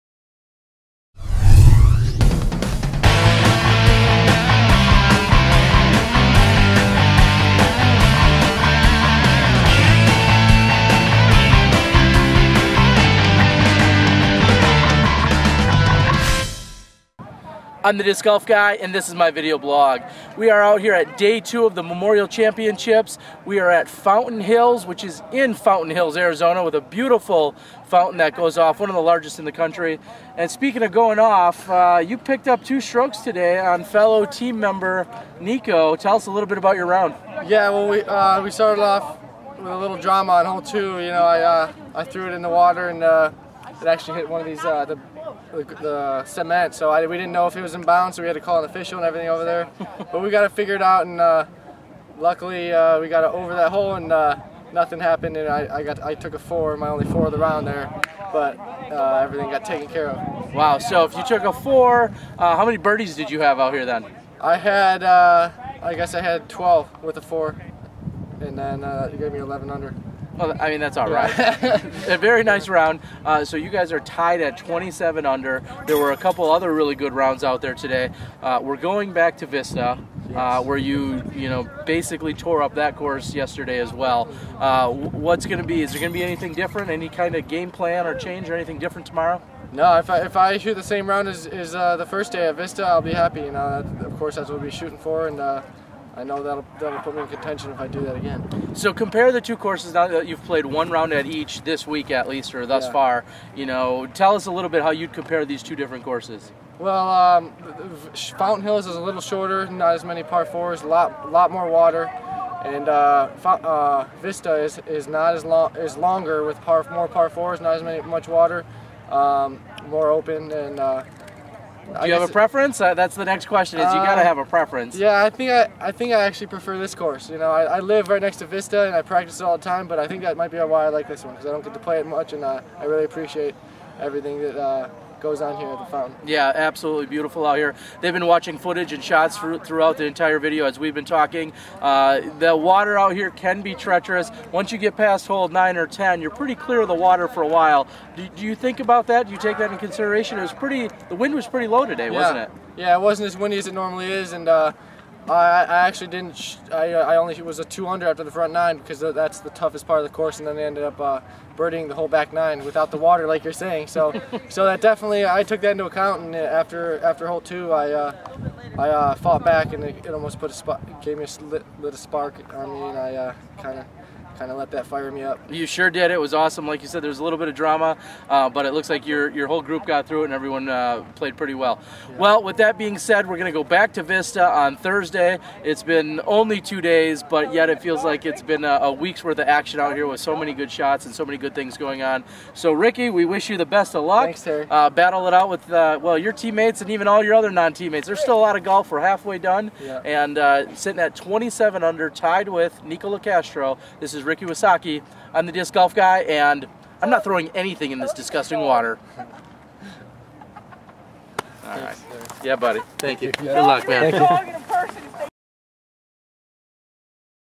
The Disc Golf Guy - Vlog #146 - Ricky Wysocki is Co-Leader at The Memorial Championships
More amazing golf took place at Fountain Hills today. Ricky has averaged approximately 1100 rated golf which has put him tied for the lead with Nikko Locastro.

2011 World Champion, Paige Pierce continued to shoot lights out as well and now has a 10 stroke lead half way through the tournament.

I hope you enjoy the highlights which include some great drives as well as some amazing putts from the beautiful Fountain Hills course.